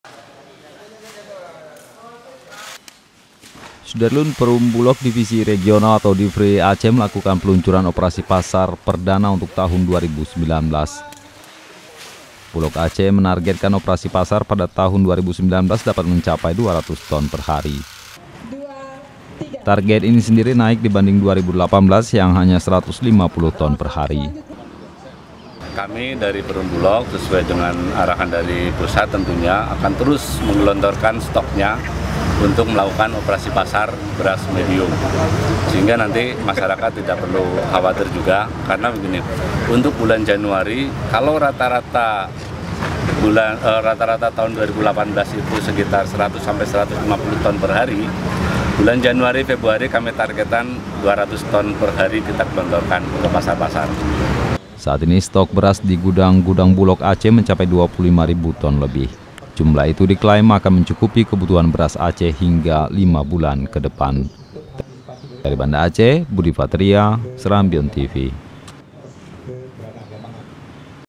Operasi Pasar 2019 Bulog Aceh Sediakan 200 Ton Beras Perhari
SERAMBITV.COM, BANDA ACEH - Perum Bulog Divisi Regional (Divre) Aceh, melakukan peluncuran Operasi Pasar perdana untuk tahun 2019, Kamis (3/1/2019).

Bulog Aceh menargetkan operasi pasar pada tahun 2019 dapat mencapai 200 ton per hari.

Target ini sendiri naik dibanding tahun 2018 yang hanya 150 ton per hari.

Saat ini stok beras di gudang-gudang Bulog Aceh mencapai 25 ribu ton lebih. Jumlah itu diklaim akan mencukupi kebutuhan beras Aceh hingga lima bulan kedepan.